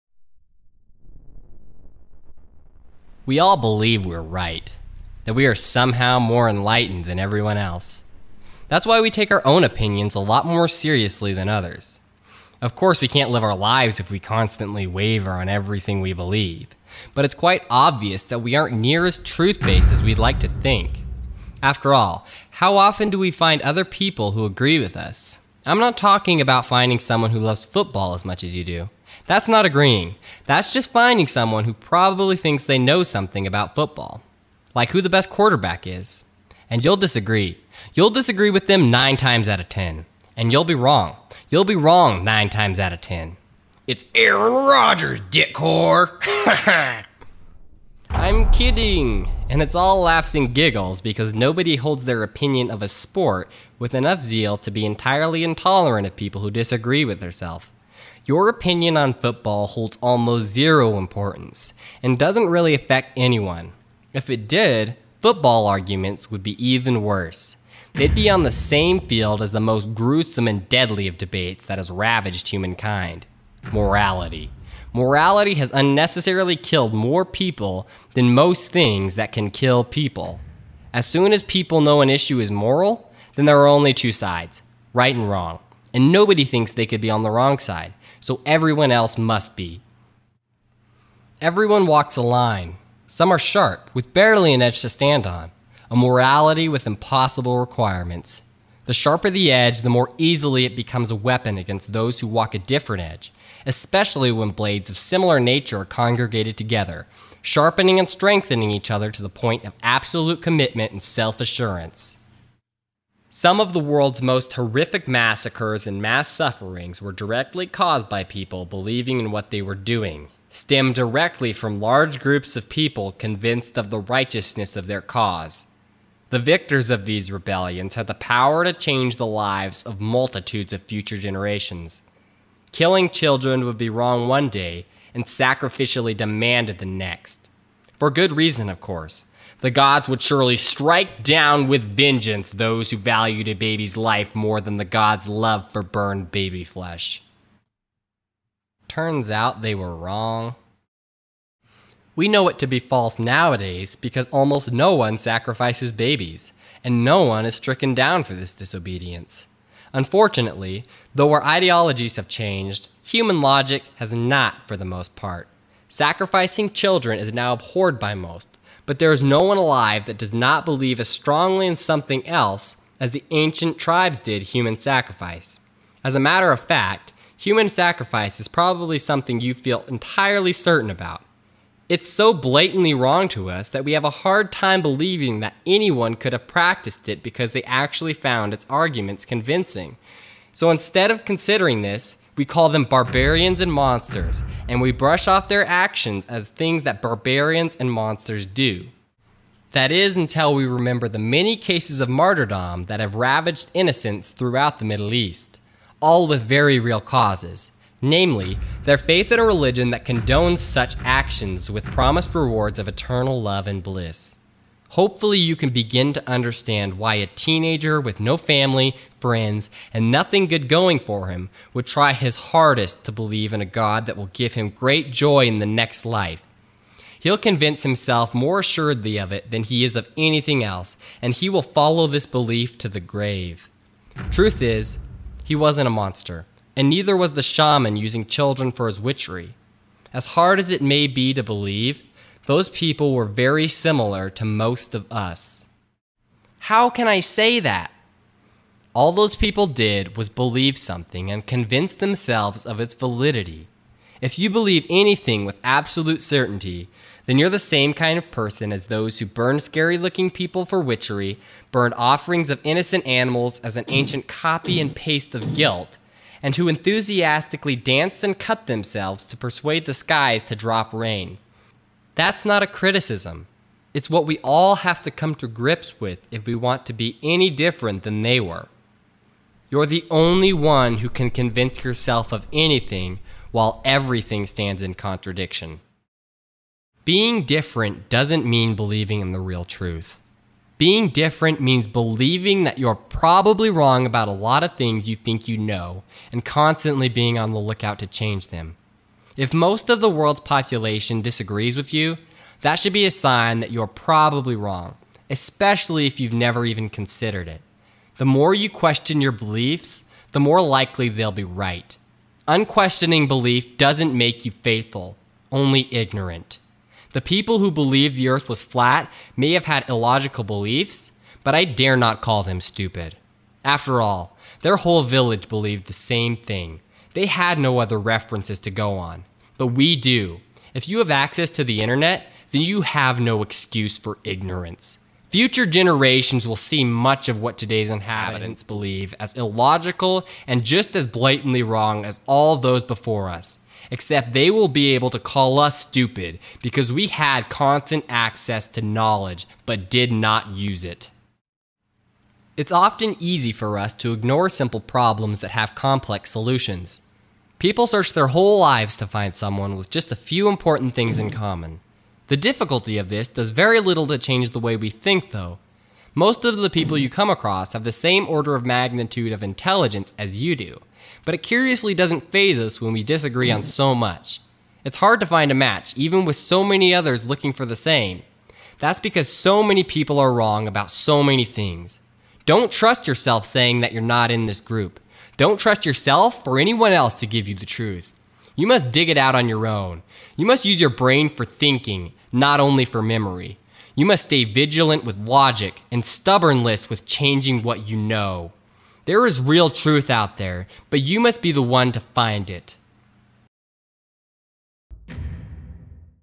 You're Probably Wrong (Statistically)
Everyone thinks that they're right, so it's no surprise that a lot of our mutually exclusive beliefs are entirely wrong and we don't even know it. So how do we tell the difference between what we believe to be true, and the truths we believe? I do my best to cover the general topic of statistical ignorance and I hope you find my video rather enjoyable.
Wanna talk to the mastermind behind this ingenious production? Better look in a different universe because I'm not and it's not, haha, but I'd still love to talk to you, so please, write me whatever's on your mind.